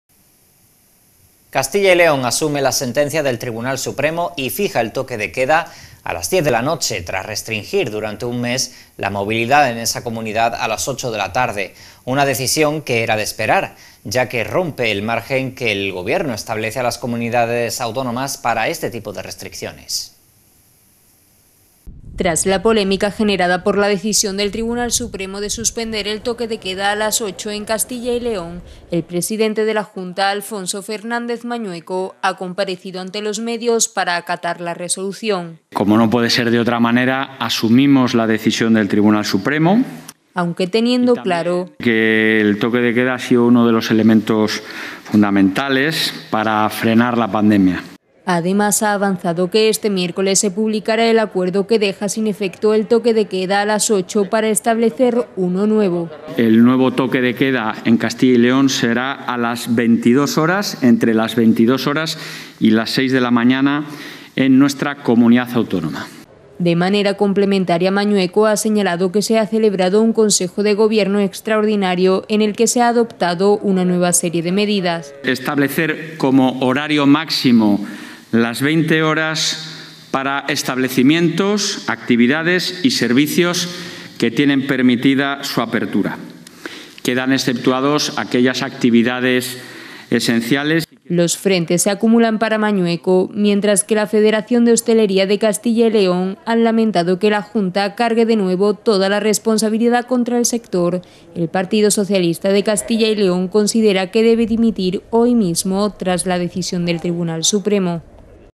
🔴Noticia - Castilla y León acata la decisión del Supremo y establece el toque de queda a las 22:00
El presidente de la Junta, Alfonso Fernández Mañueco, ha avanzado que este miércoles, 17 de febrero, se publicará el acuerdo que deja sin efecto el toque de queda a las 20.00 horas para establecerse entre las 22.00 y las 6.00 horas. De manera complementaria, Fernández Mañueco ha señalado que se ha celebrado un Consejo de Gobierno Extraordinario en el que se han adoptado una serie de medidas "para favorecer el aislamiento entre personas, reducir contactos y evitar contagios", por lo que se establece como horario máximo las 20.00 horas para establecimientos y servicios que tienen permitida su apertura, a excepción de las actividades esenciales. -. Firma: RAQU/MLUZ .- (Fuente: Europa Press y Junta CyL)

English:
The Chairman of the Board, Alfonso Fernández Mañueco, has advanced that this Wednesday, February 17, the agreement will be published that nullifies the curfew at 8:00 p.m. to be established between 10 p.m. and 6 a.m. In a complementary way, Fernández Mañueco has indicated that an Extraordinary Government Council has been held in which a series of measures have been adopted "to promote isolation between people, reduce contacts and avoid contagion", which is why it is established as a maximum time 8:00 p.m. for establishments and services that are allowed to open, except for essential activities. -. Signature: RAQU / MLUZ .- (Source: Europa Press and Junta CyL)

Live streaming